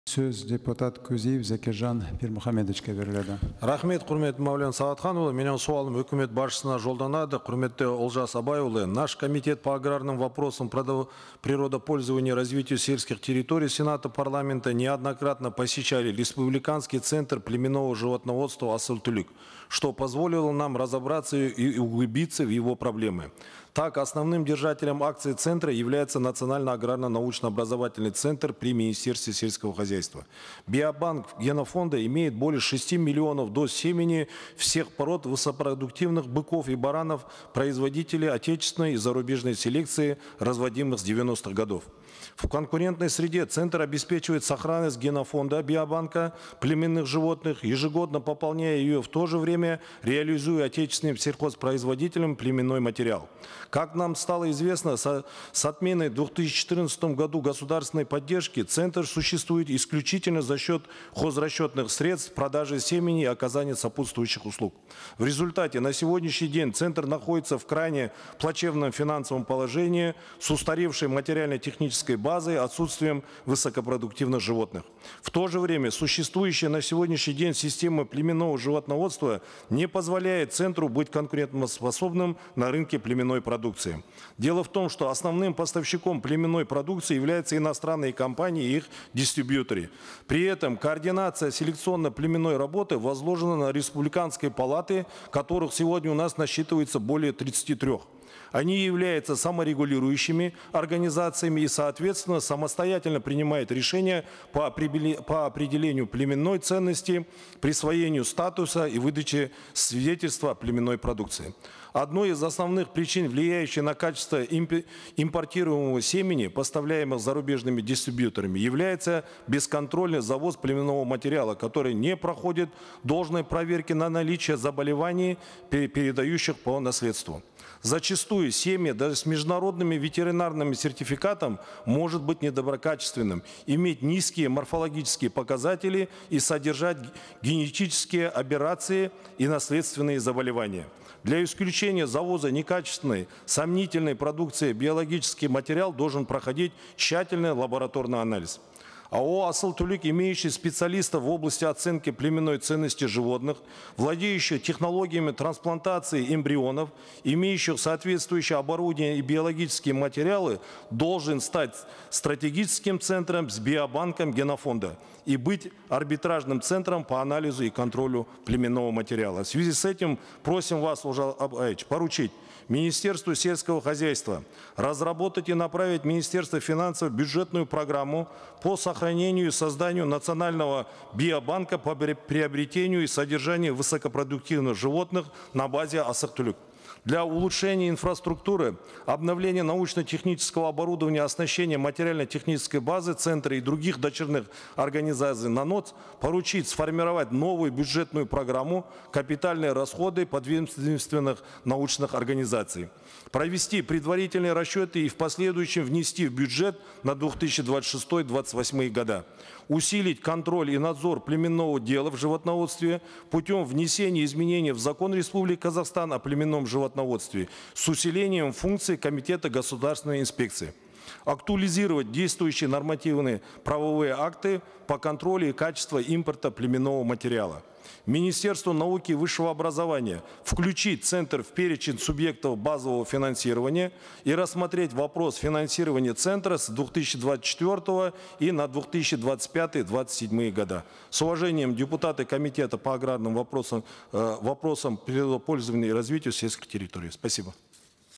Закиржан Кузиев призывает усилить поддержку центра «Асыл Түлік»
Закиржан Кузиев «Асыл Түлік» орталығын қолдауды күшейтуге шақырды: Асыл тұқымды мал шаруашылығын дамыту және генофондты сақтау керек